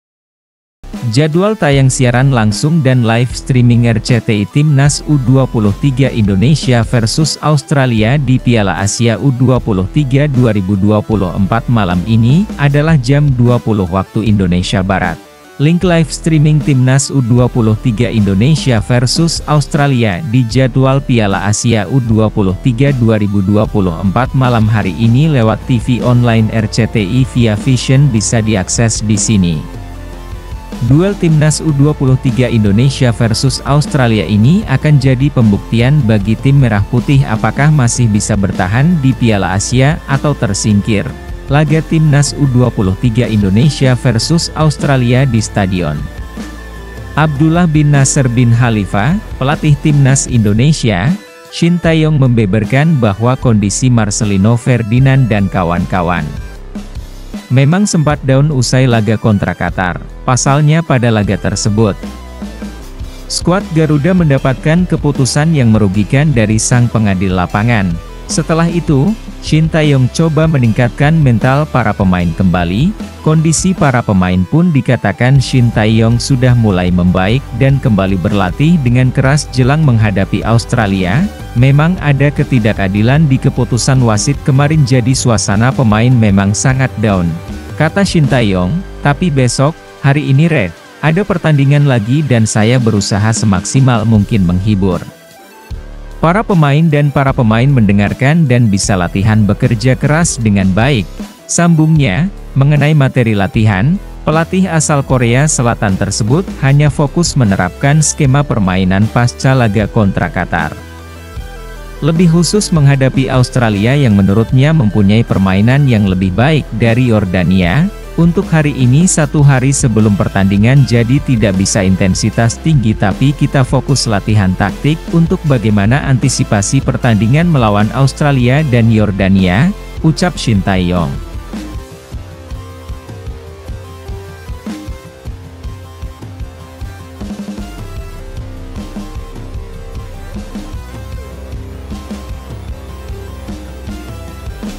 🔴 SEDANG BERLANGSUNG !! Timnas Indonesia Vs Australi Piala Asia Piala Asia U23 2024 (ilustrasi)
Jadwal tayang siaran langsung dan Live Streaming RCTI Timnas U23 Indonesia vs Australia di Piala Asia U23 2024 malam ini adalah jam 20.00 WIB.
Link Live Streaming Timnas U23 Indonesia vs Australia di jadwal Piala Asia U23 2024 malam hari ini lewat TV Online RCTI via Vision + bisa diakses di sini.
Duel Timnas U23 Indonesia vs Australia ini akan jadi pembuktian bagi tim Merah Putih apakah masih bisa bertahan di Piala Asia atau tersingkir.
Laga Timnas U23 Indonesia vs Australia di Stadion Abdullah bin Nasser bin Khalifa.
Pelatih Timnas Indonesia, Shin Tae-yong membeberkan bahwa kondisi Marselino Ferdinan dkk. memang sempat down usai laga kontra Qatar.
Pasalnya pada laga tersebut, Skuad Garuda mendapatkan keputusan yang merugikan dari sang pengadil lapangan.
Setelah itu, Shin Tae-yong coba meningkatkan mental para pemain kembali.
Kondisi para pemain pun dikatakan Shin Tae-yong sudah mulai membaik dan kembali berlatih dengan keras jelang menghadapi Australia.
“Memang ada ketidakadilan di keputusan wasit kemarin jadi suasana pemain memang sangat down,” kata Shin Tae-yong.
“Tapi besok (hari ini-red) ada pertandingan lagi dan saya berusaha semaksimal mungkin menghibur para pemain dan para pemain mendengarkan dan bisa latihan bekerja keras dengan baik,” sambungnya.
Mengenai materi latihan, pelatih asal Korea Selatan tersebut hanya fokus menerapkan skema permainan pasca laga kontra Qatar.
Lebih khusus menghadapi Australia yang menurutnya mempunyai permainan yang lebih baik dari Yordania.
“Untuk hari ini satu hari sebelum pertandingan jadi tidak bisa intensitas tinggi tapi kita fokus latihan taktik untuk bagaimana antisipasi pertandingan melawan Australia dan Yordania,” ucap Shin Tae-yong.

Jika kalian suka jangan lupa suport Chanel ini dengan cara :
√ Coment     √Share